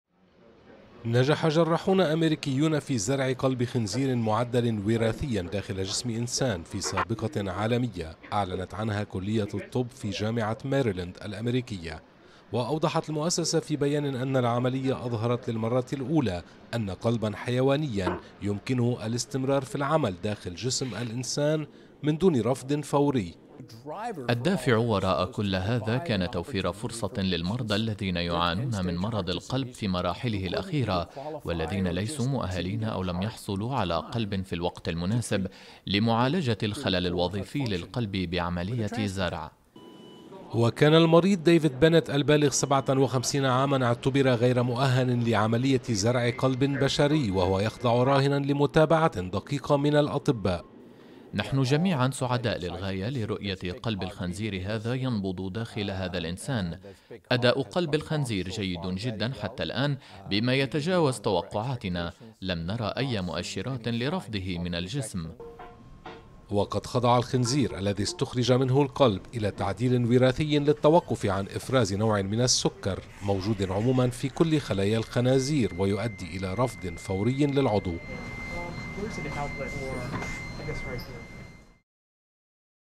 جراحون في مستشفى أميركي ينجحون بزرع قلب خنزير في جسم إنسان | AFP
قلب الحيوان يمكنه أن يعمل داخل جسم الإنسان... هذا ما أعلنته كلية الطب في جامعة ميريلاند بعد زرع قلب خنزير لأحد المرضى في الولايات المتحدة.